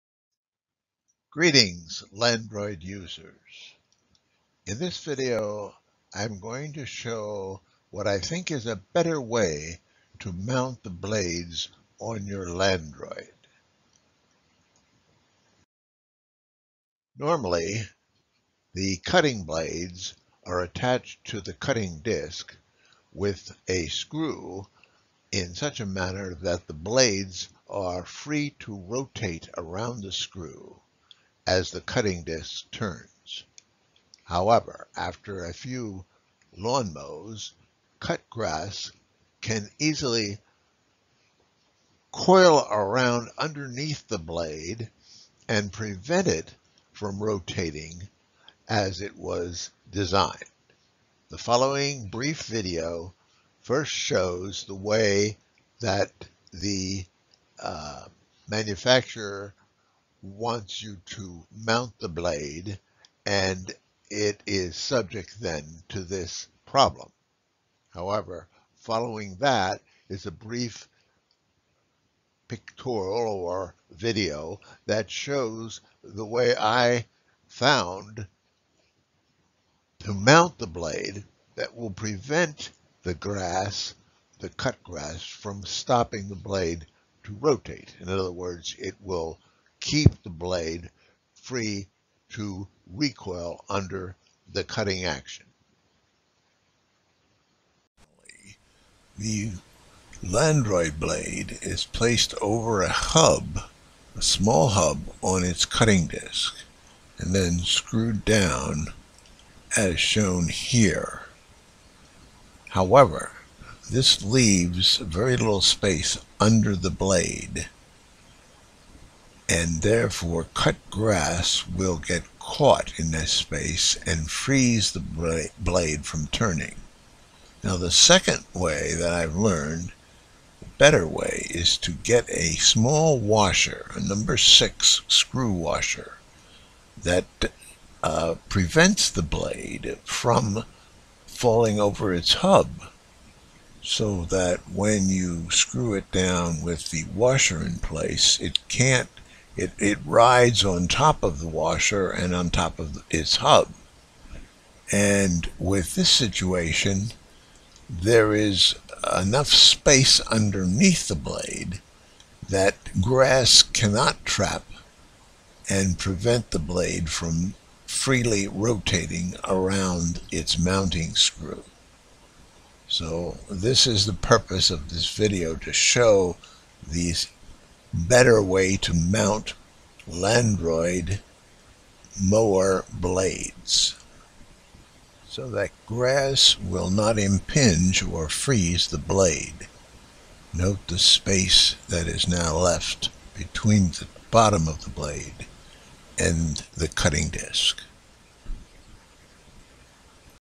Landroid - A Better Blade Mount
The existing Landroid blade mount is flawed.   After a few lawn mows, cut grass can accumulate beneath the blades.  This prevents the blade/s from being able to rotate around their mounting screw, as designed.   After some experimentation, I was able to find a way to mount these blades so that their freedom to rotate is always maintained.